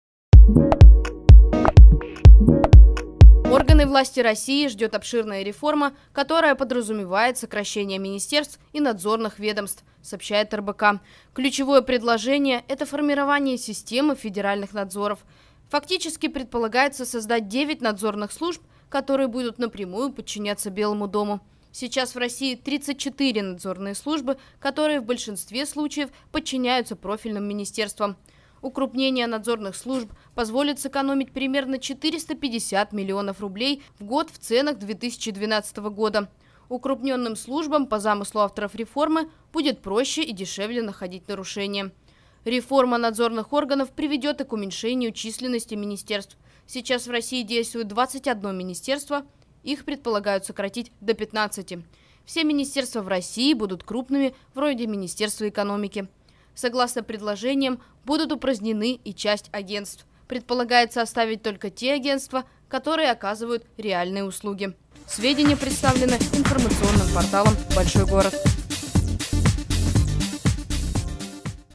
В России могут сократить количество чиновников
Чиновники обсуждают крупную реформу госорганов. Если она будет проведена, радикально сократится число министерств и надзорных органов. Все они будут укрупнены.